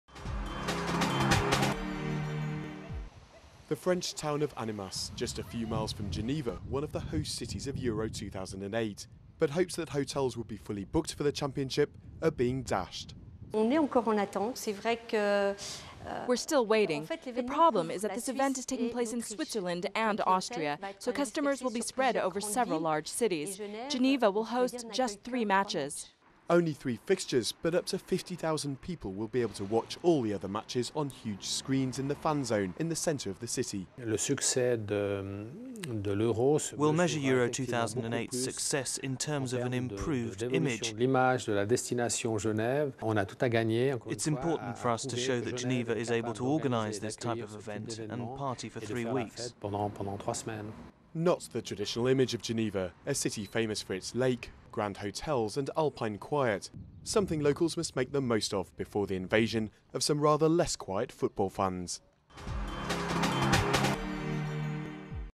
EURO 2008 - Tourism & economy
There's nothing like a football tournament to raise a host country's profile. Geneva, where some of the Euro 2008 matches are being held, is hoping to cash in on the championship fever to become the holiday destination of choice.